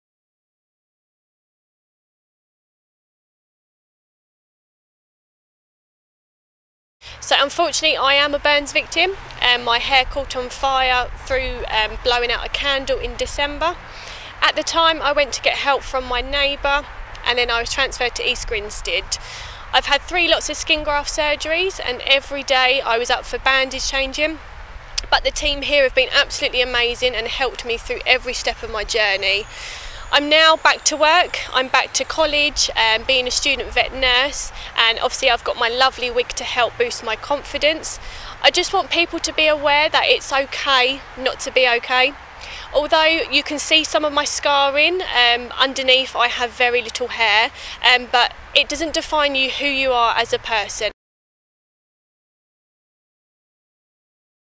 Emily’s candle warning
In the lead-up to the festive season, experts at Queen Victoria Hospital (QVH) have been joined by one of their patients to remind people to be burns aware, especially in the home.